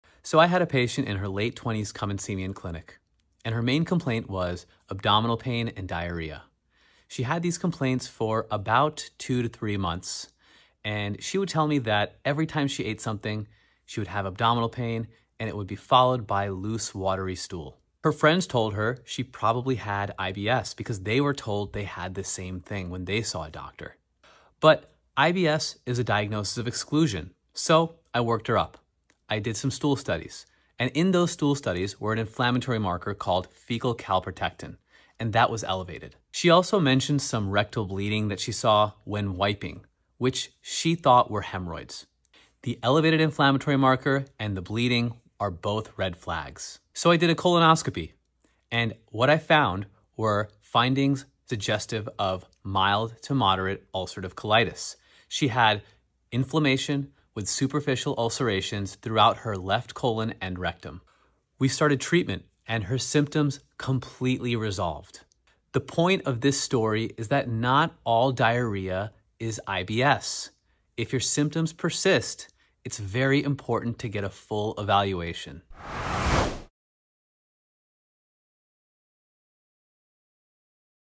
Is Your ‘IBS’ Actually Ulcerative Colitis? (Symptoms Doctors Don’t Ignore)
Most people assume diarrhea and abdominal pain are “just IBS.”
But IBS is a diagnosis of exclusion, and sometimes the real cause is inflammation.

This 28-year-old woman had:
• post-meal urgency
• abdominal cramping
• loose stools
• rectal bleeding she thought was hemorrhoids
• 3 months of persistent symptoms

Her stool test (fecal calprotectin) was elevated.
Her colonoscopy showed superficial ulcerations in the rectum and left colon, classic ulcerative colitis.

After treatment, her symptoms resolved completely.

If your IBS-like symptoms keep coming back, get evaluated for IBD.